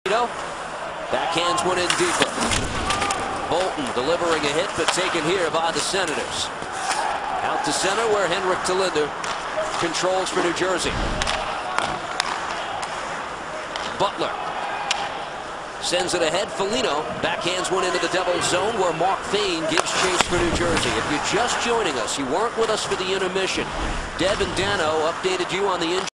Zenon Konopka hits Mark Fayne
Hit in a game between the New Jersey Devils and Ottawa Senators on Thursday, December 8th, 2011 at the Prudential Center in Newark, New Jersey.